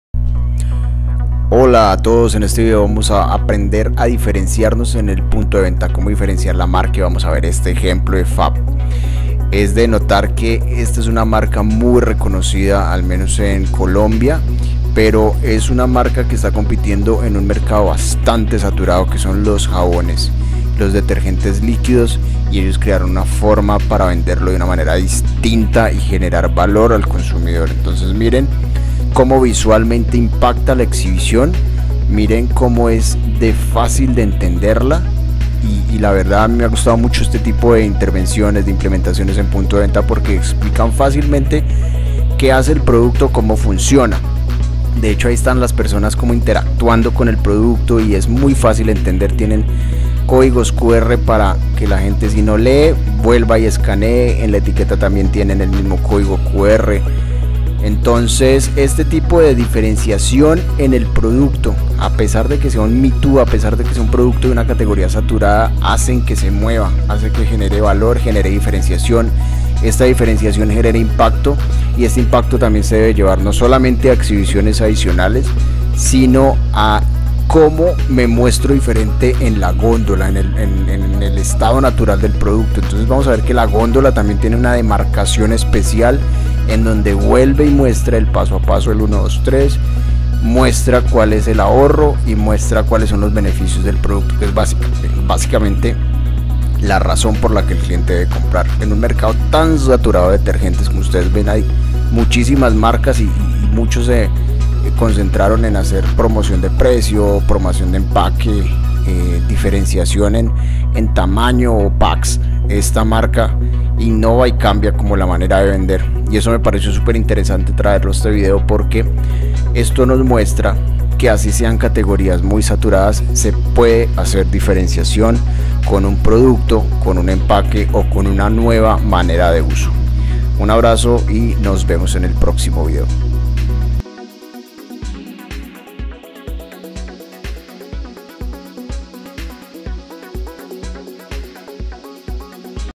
¿COMO DIFERENCIAR LA MARCA EN LA TIENDA?
Para diferenciarse en el punto de venta debe ser obligatorio cumplir con 3 cosas: Impacto, atracción y comunicación clara.